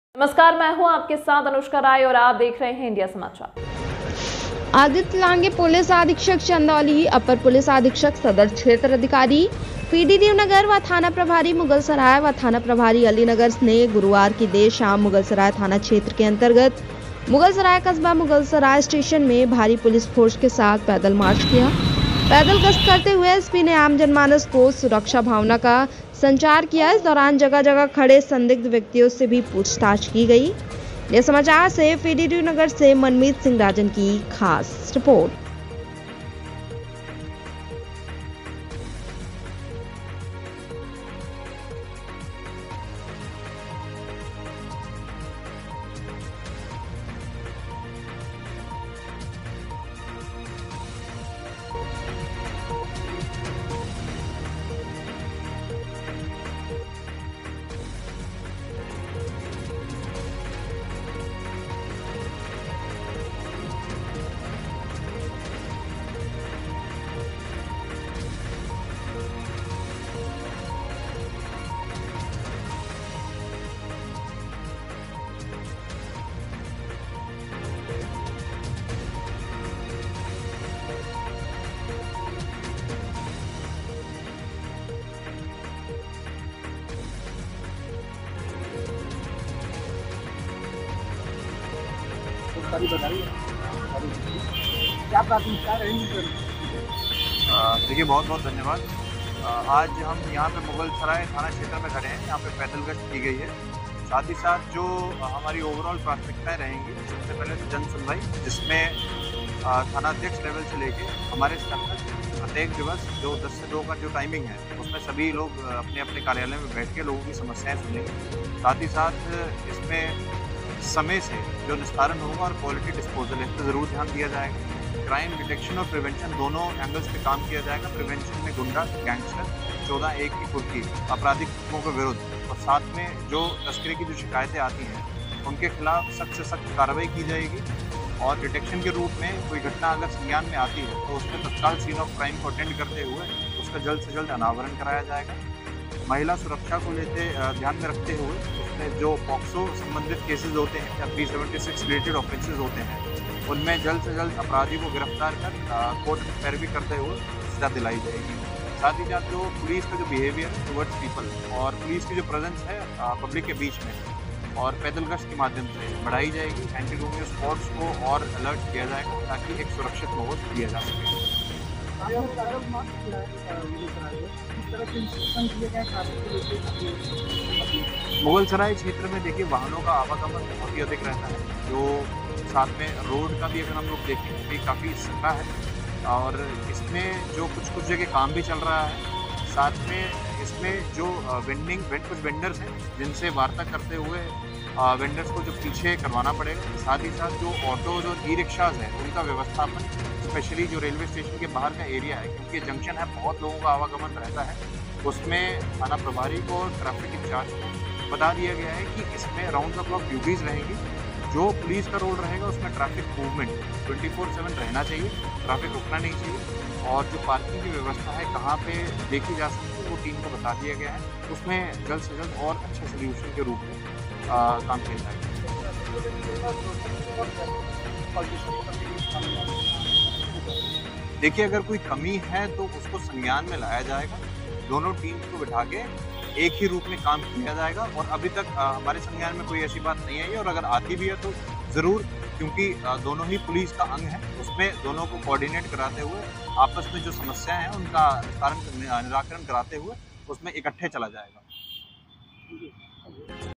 मुगलसराय थाना क्षेत्र के कस्बे में किया गया पैदल गश्त
आदित्य लांग्हे पुलिस अधीक्षक चन्दौली, विनय कुमार अपर पुलिस अधीक्षक सदर व अनिरूद्ध सिंह क्षेत्राधिकारी पीडीडीयू नगर द्वारा मुगलसराय थाना क्षेत्र के कस्बे में किया गया पैदल गश्त*

*चन्दौली-* आदित्य लांग्हे पुलिस अधीक्षक चन्दौली ,अपर पुलिस अधीक्षक सदर,क्षेत्राधिकारी पीडीडीयू नगर व थाना प्रभारी मुगलसराय व थाना प्रभारी अलीनगर ने गुरुवार की देर शाम मुगलसराय थाना क्षेत्र अन्तर्गत मुगलसराय कस्बा, मुगलसराय स्टेशन में भारी पुलिस फोर्स के साथ पैदल मार्च किया। पैदल गश्त करते हुए एसपी ने आम जनमानस में सुरक्षा भावना का संचार किया। इस दौरान जगह-जगह खड़े संदिग्ध व्यक्तियों से पूछताछ भी की गई। एसपी ने सभी को चेताया कि आप कोई भी उपद्रव ना करें। अगर कोई ऐसा करता है तो पुलिस को सूचित करें, पुलिस आपका सहयोग करेगी। आपकी सतर्कता से न सिर्फ अपराध रुकेगा बल्कि क्षेत्र में शांति भी रहेगी।              पिडीडियू नगर से मनमीत सिंह राजन की रिपोर्ट